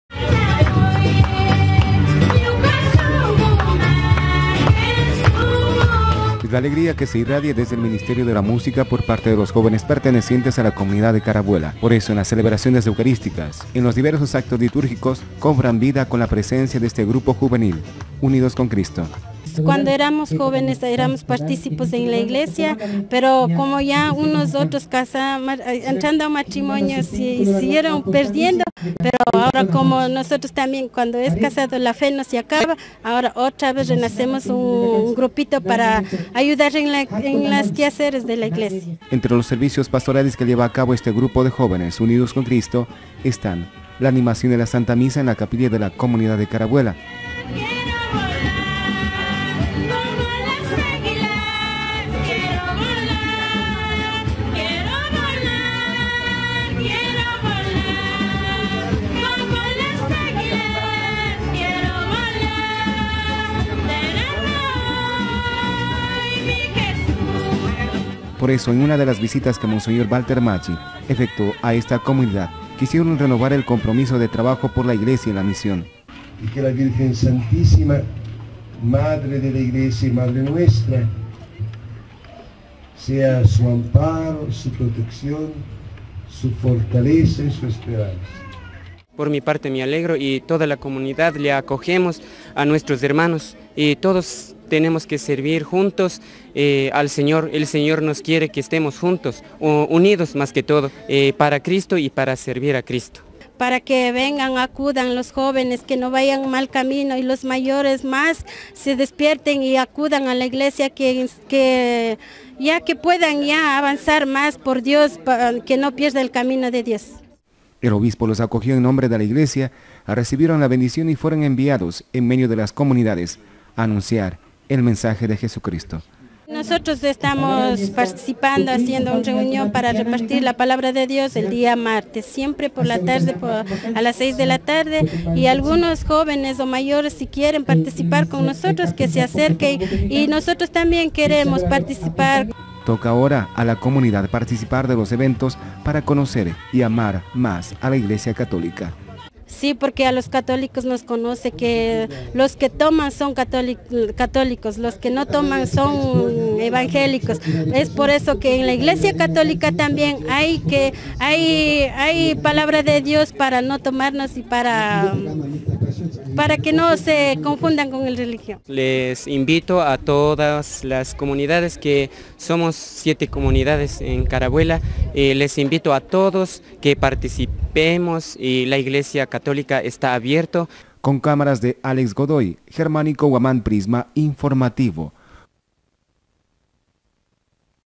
Jóvenes de Carabuela, se comprometieron a trabajar en el campo de la animación y Evangelización
Hace pocos días jóvenes de la comunidad de Carabuela, se comprometieron en la Iglesia para llevar adelante la acción misionera y la animación litúrgica. Tanto la comunidad como los catequistas, hacen un llamado a la juventud a ser parte activa dentro en el caminar de la comunidad cristiana.